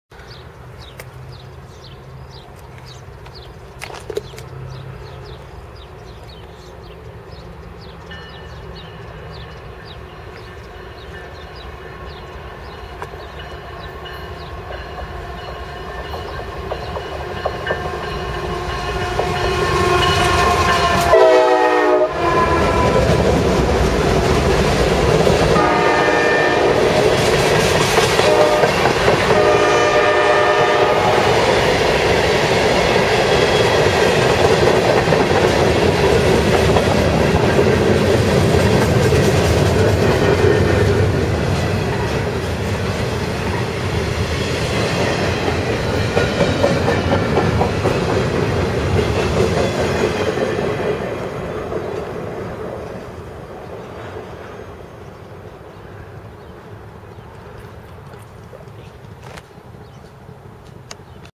Iowa Interstate Railroad - New Lenox Illinois. June 7th, 2010
Iowa Interstate Railroad's Westbound road freight for Council Bluffs rolls through New Lenox Illinois, 34 miles Southwest of Chicago on Metra's Rock Island district main line, a very short train this evening.